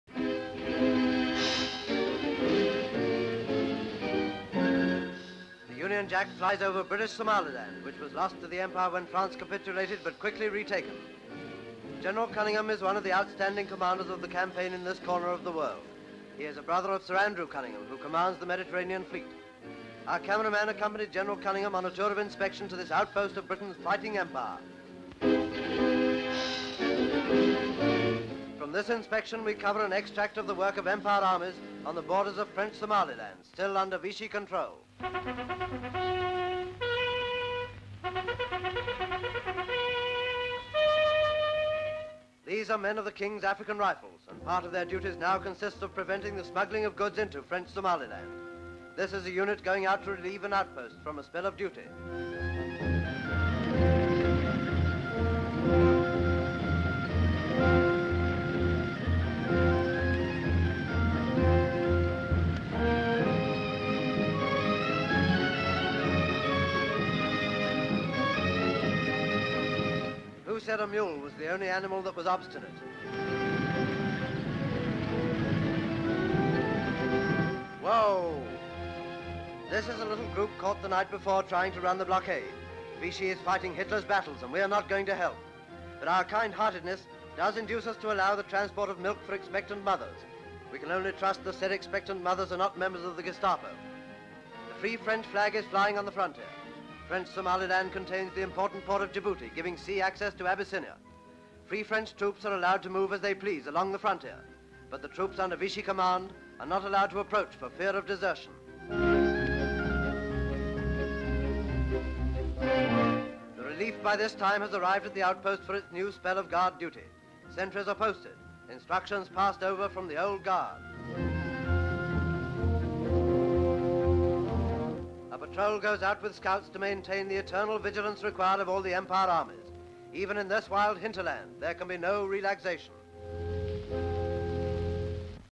Empire armys on the border in British Somaliland (1941)
General Cunningham inspects troops at outpost of Somaliland

Full Description:

SLATE INFORMATION: With The Army In British Somaliland

REPUBLIC OF SOMALILAND:
EXT

COX CAPTAIN. (O.C.Commanding in British Somaliland). Greets General Cunninham on his arrival by plane to advanced posts

CUNNINGHAM GENERAL. Sir Alan. With the Army in British Somaliland..arrives in plane..visits H.Q..then Free French Senegalese.

FLAGS. Free French flag flies over the frontier in British Somaliland

MILITARY BRITISH. Gen. Cunningham visits British Somaliland..arrives in plane..King's African Rifles go out to relieve outpost..visits Free French Senegalese..F.F.flag flying on frontier..soldiers pass Djibouti frontier..sentries posted & instructions given to old guard..patrols go out etc.

Somalia; Military - Active

troops, infantry, infantrymen, World War II, WWII, French, France,
Background: General Cunningham inspects troops at outpost of Somaliland